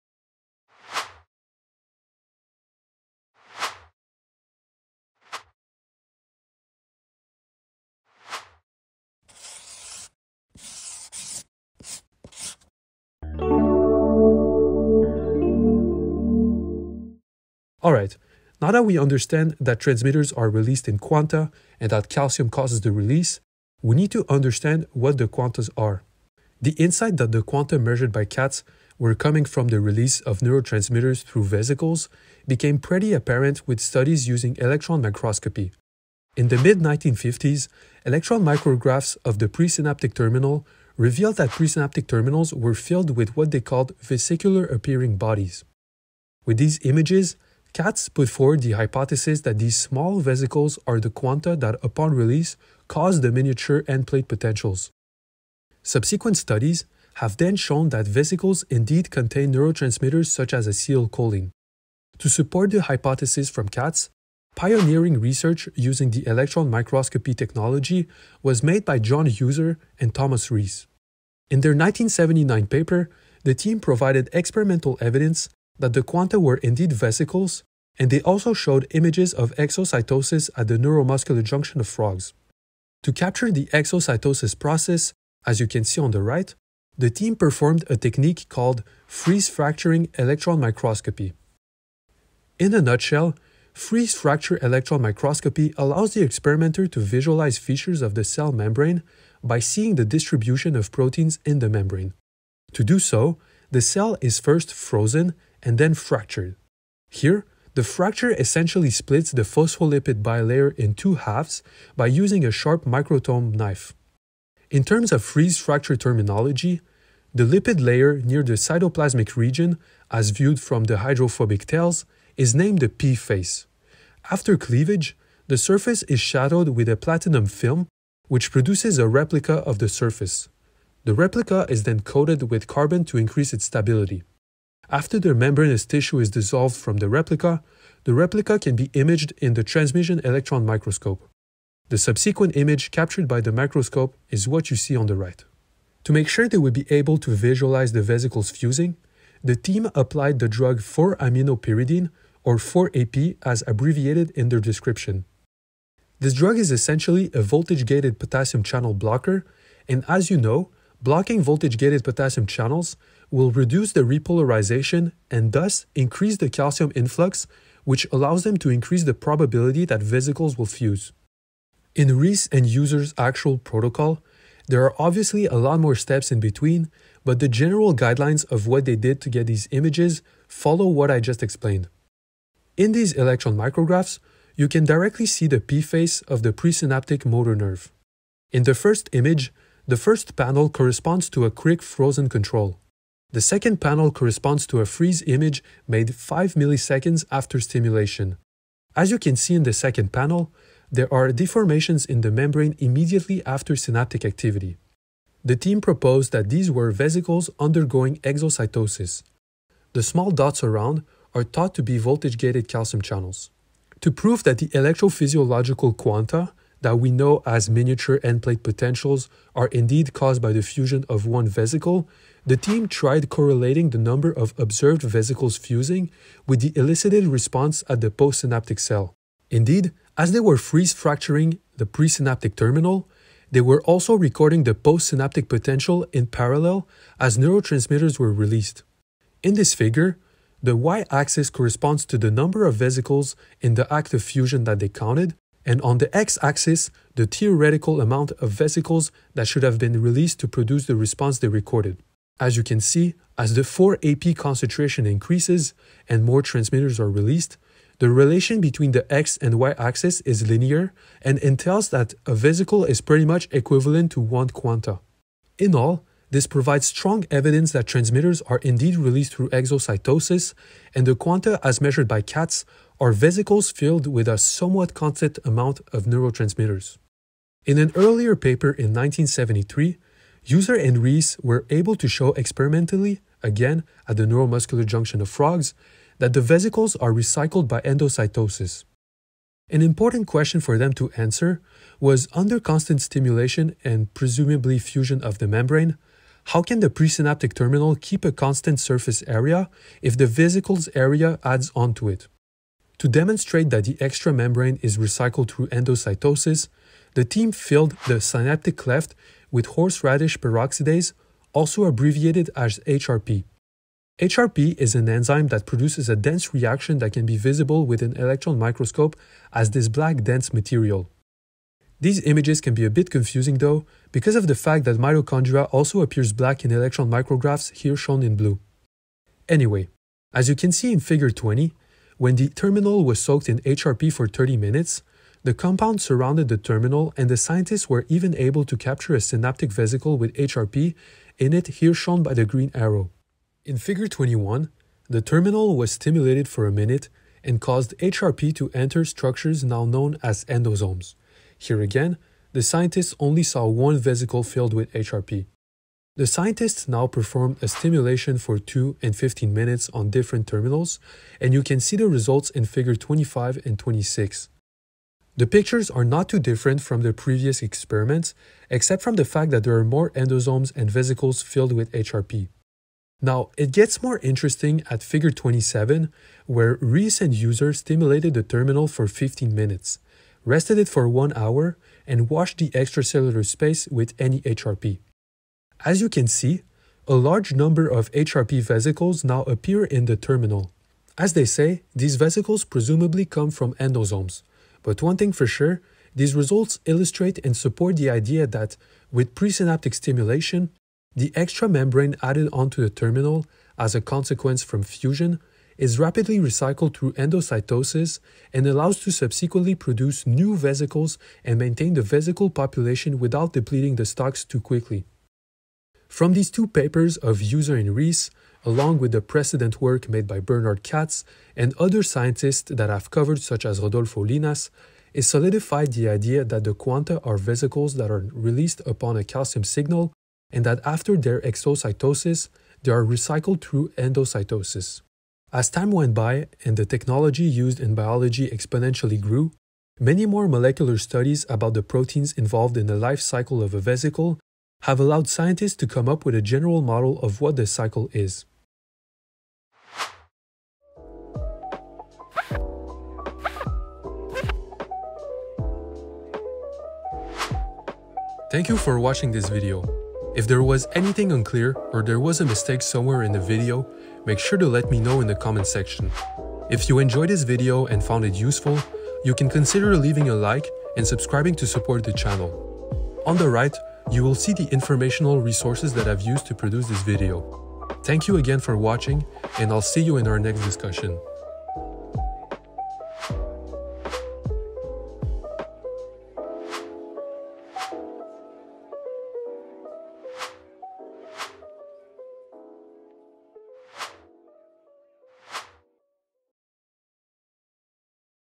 Experimental Basis for Exocytosis & Endocytosis (Freeze-Fracture Electron Microscopy) | Clip
Welcome to Science With Tal!

In this clip of the The Neuromuscular Junction as the model of the chemical synapse video, we will cover the experimental basis for the discovery of vesicle release (exocytosis) and the retrieval of the vesicles (endocytosis). The two experiments presented in this video come from John Heuser and Tom Reese. The video will also cover topics such as freeze-fracture electron microscopy.

To improve the quality of my content, I highly value the feedback from the viewer so do not hesitate to give any feedback in the comment section.

Timestamps

0:00 Introduction
0:16 First evidence of vesicles (1956)
1:02 Heuser & Reese exocytosis experiment (1979)
1:22 Freeze fracture electron microscopy & interpretation of results
4:38 Heuser & Reese endocytosis experiment (1973)
4:15 Quantal theory of release & statistical proof
7:30 Conclusion

Resources used

Here is a list of the resources that I’ve used to produce this video. (Author: title resource)

- Dale Purves: Neuroscience (6th edition)
- Eric Kandel: Principles of neural science (6th edition)
- Robertson, J. David: Some Features of the Ultrastructure of Reptilian
Skeletal Muscle
- Heuser, J E et al. : Synaptic vesicle exocytosis captured by quick freezing
and correlated with quantal transmitter release
- Heuser, J E and T S Reese: Evidence for recycling of synaptic vesicle
membrane during transmitter release at the frog neuromuscular junction
- Llinás, R and Y Yarom: Properties and distribution of ionic
conductances generating electroresponsiveness of mammalian inferior olivary neurones in vitro
- Katz, B, and R Miledi: A study of synaptic transmission in the
absence of nerve impulses
- BOYD, I A,and A R MARTIN: The end-plate potential in mammalian
muscle
- DEL CASTILLO, J, and B KATZ: Quantal components of the end-plate potential

To have more information on these resources, you can refer to the conclusion section where a more formal citation is provided.

Video credits